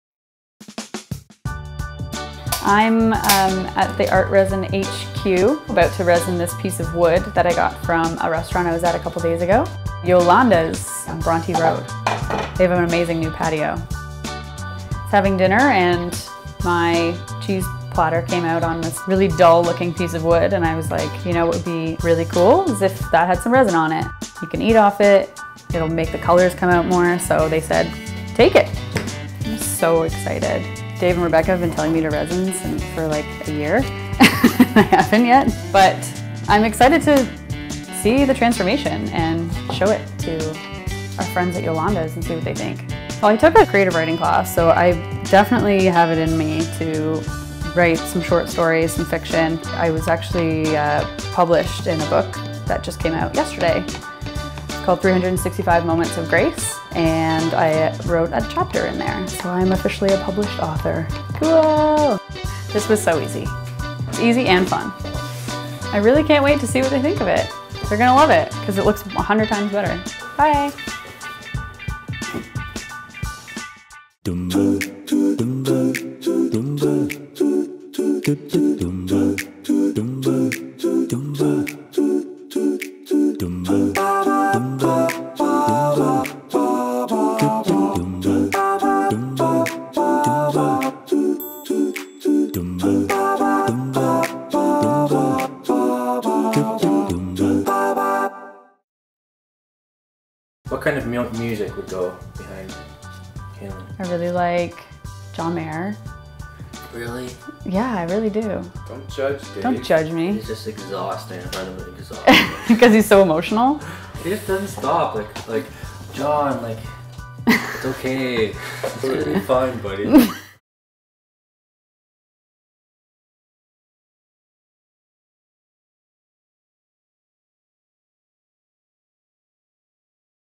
DIY wood slice serving tray
ArtResin is food safe epoxy resin so, once cured, you can eat off of it.  Kaylyn took a lovely slice of wood that was used as a serving tray, and made it ultra-extra lovely with a coat of ArtResin.  You can eat off it and it makes the colors pop!  Kaylyn has been working with the ArtResin Canada team for a few months, pursuing her passion as a writer, but this was her first time actually using ArtResin!  She did great, and the tray turned out great :)  Thanks KG!!! :)

Easy Epoxy Resin Coating Wood Serving Tray.